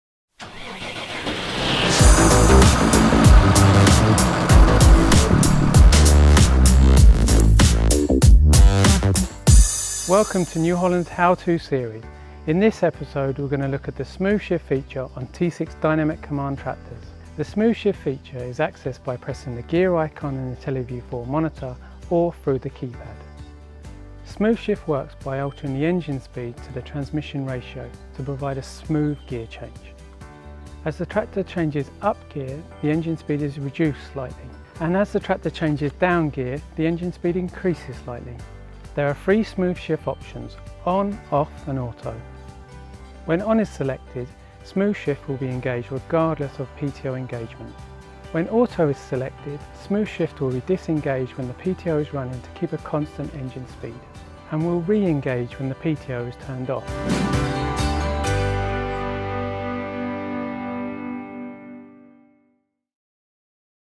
T6 Dynamic Command™ - Smooth shift feature
New Holland "How To" video series: in this episode we are going to look at how to use the smooth shift feature on T6 Dynamic Command™ tractors.